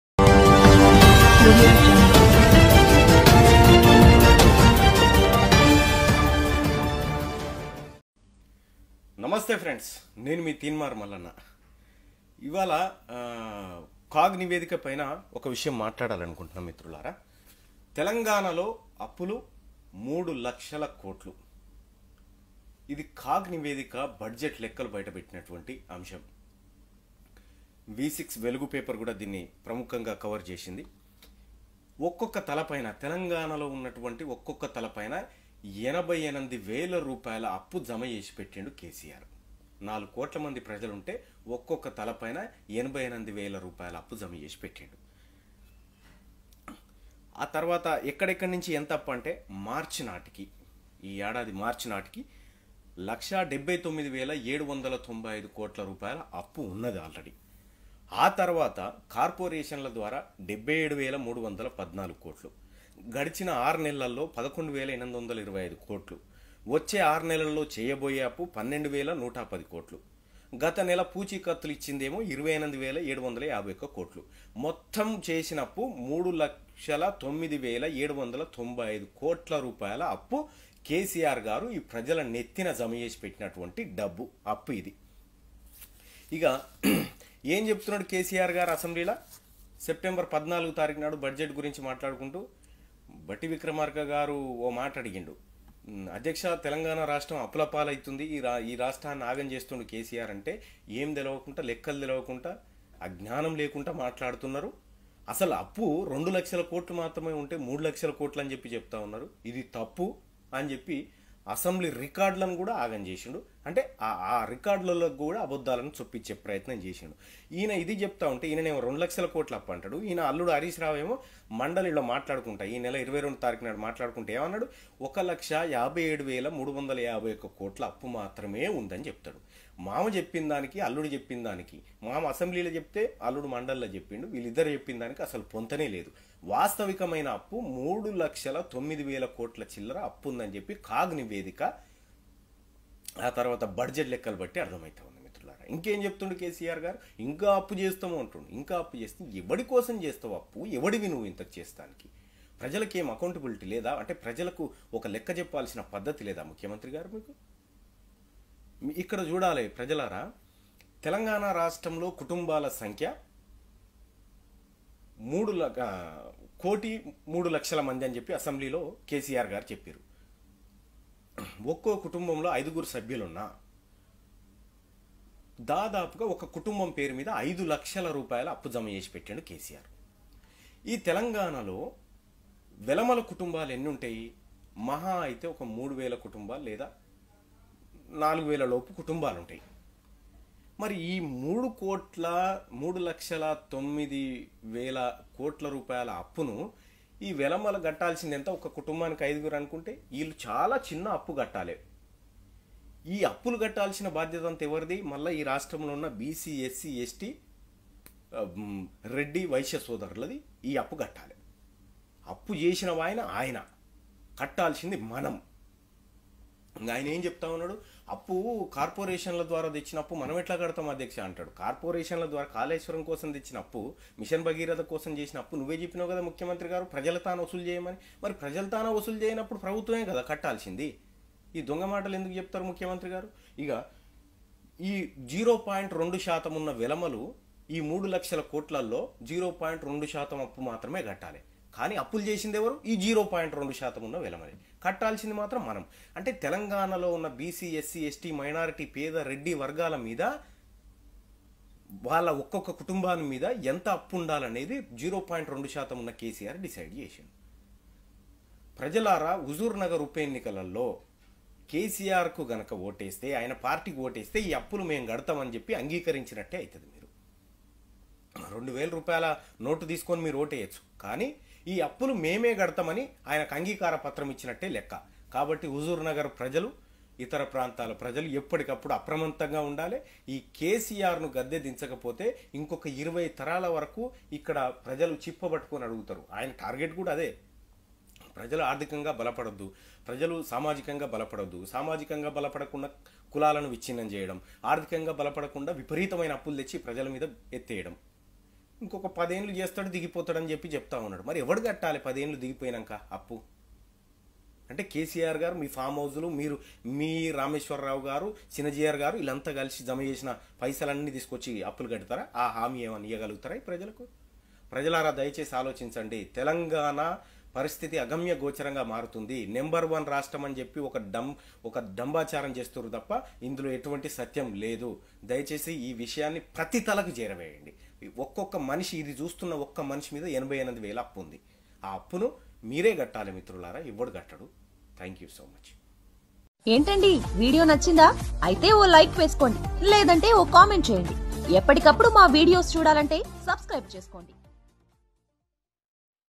అప్పుల వెనుక అబద్ధాలు తెలంగాణ అప్పులు 3లక్షల కోట్లు నిధులను బట్టబయలు చేసిన తీన్మార్ మల్లన | FFN
Watch#